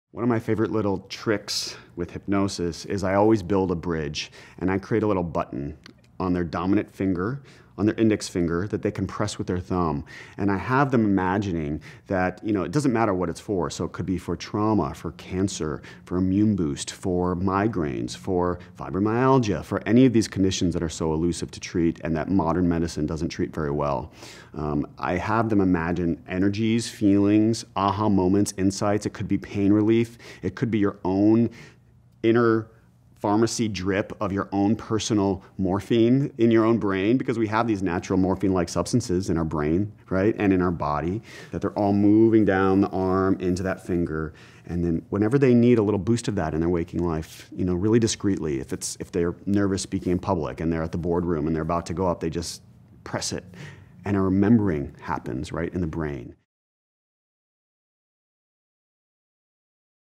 Dr. Mike Dow - Hypnosis Trick For Chronic Illness
Want to learn a quick hypnosis trick for chronic illness?  Check out this clip from our Hay House HEAL Summit featuring psychotherapist and New York Times bestselling author,  Dr. Mike Dow.

Inspired by his brother who suffered a massive stroke when he was just 10 years old, Dr. Mike made it his personal mission to help people heal their brains.  In his most recent book, Chicken Soup for the Soul’s Think, Act & Be Happy: How to Use Chicken Soup for the Soul Stories to Train Your Brain to Be Your Own Therapist, Dr. Mike helps people train their brains with the power of cognitive behavioral therapy.

See where you can watch HEAL Documentary now by going

Read The HEAL Book with double the content of the movie